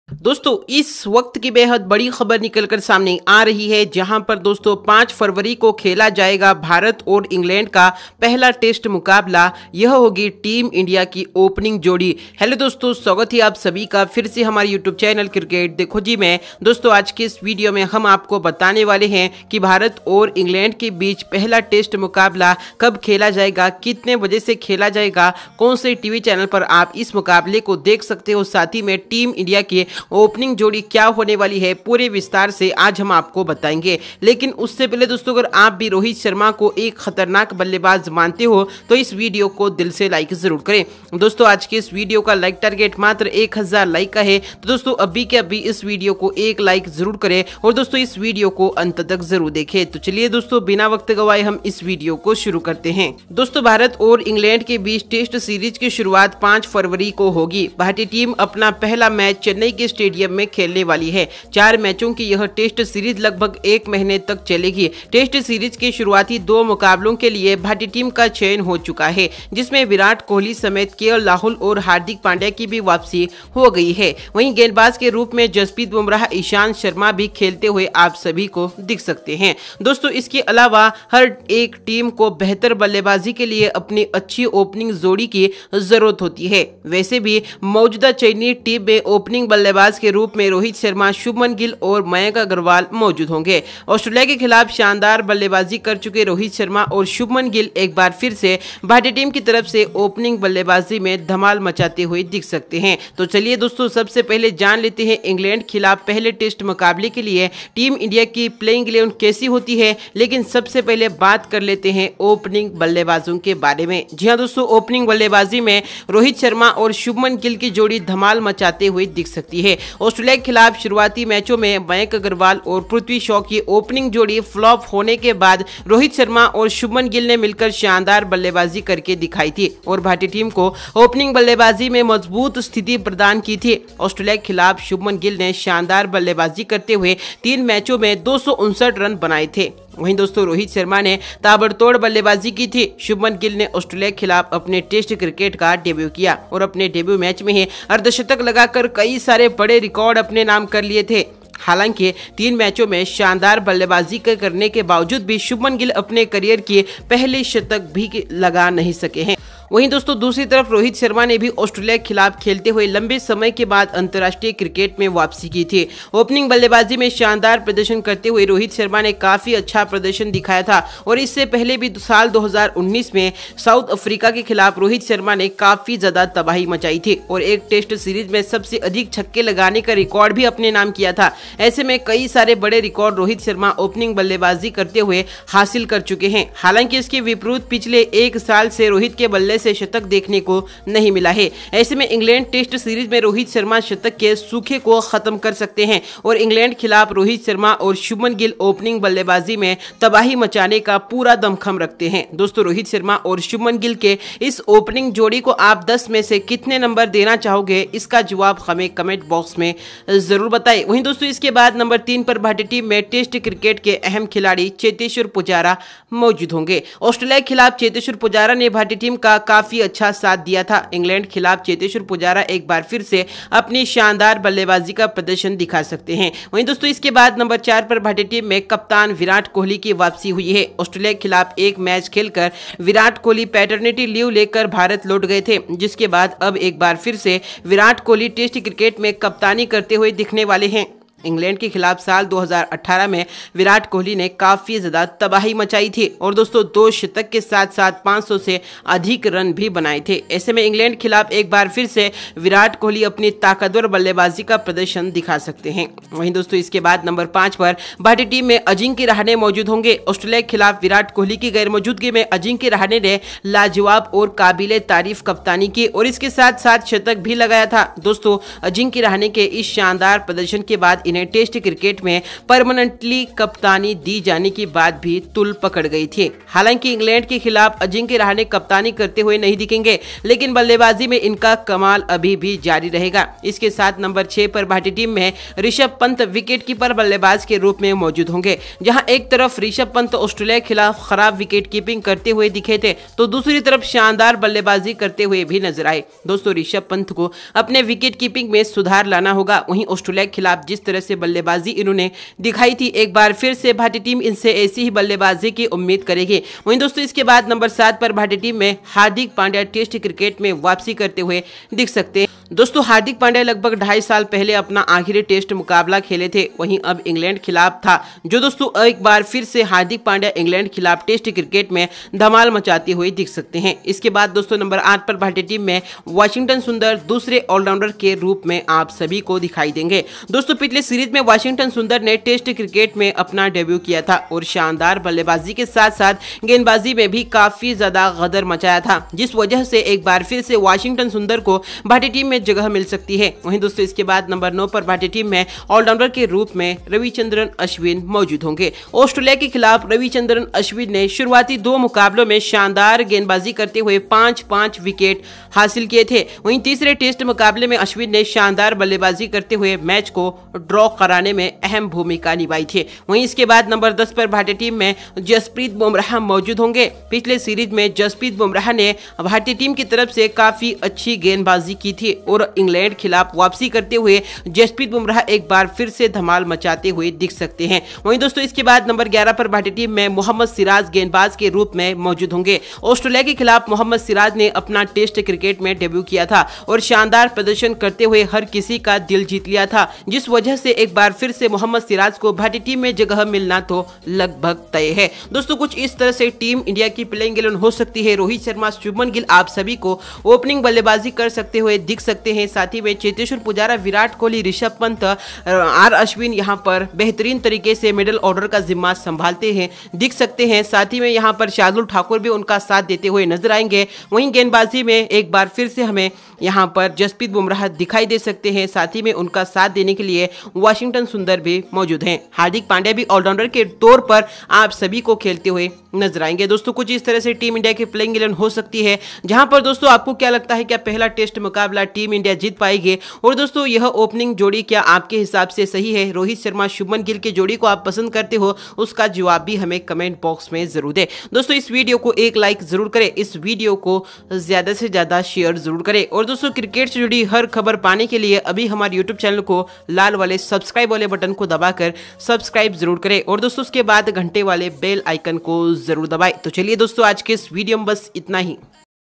5 फरवरी को खेला जाएगा भारत और इंग्लैंड का पहला टेस्ट मुकाबला, यह होगी टीम इंडिया की ओपनिंग जोड़ी
5 फरवरी को खेला जाएगा भारत और इंग्लैंड का पहला टेस्ट मुकाबला, यह होगी टीम इंडिया की ओपनिंग जोड़ी